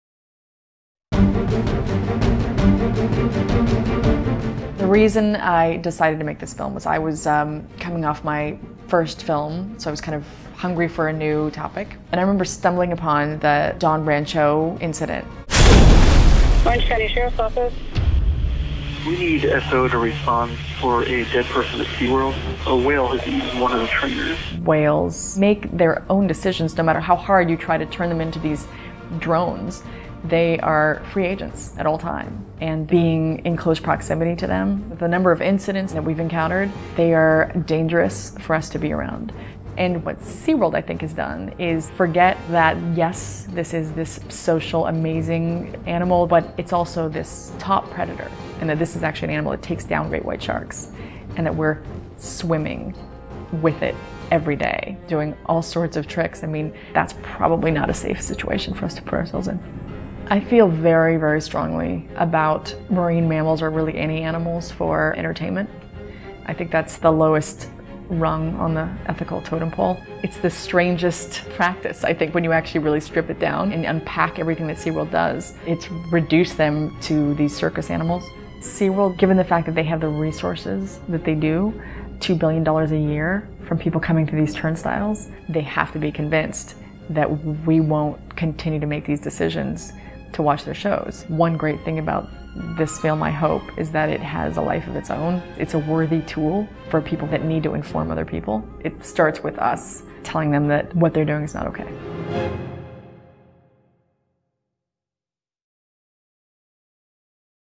Gabriela Cowperthwaite 'Blackfish' Interview
Get a sneak peek at "Blackfish," the eye-opening documentary exposé on SeaWorld and captive orcas, and meet the film's director, Gabriela Cowperthwaite, in this exclusive interview for PETA.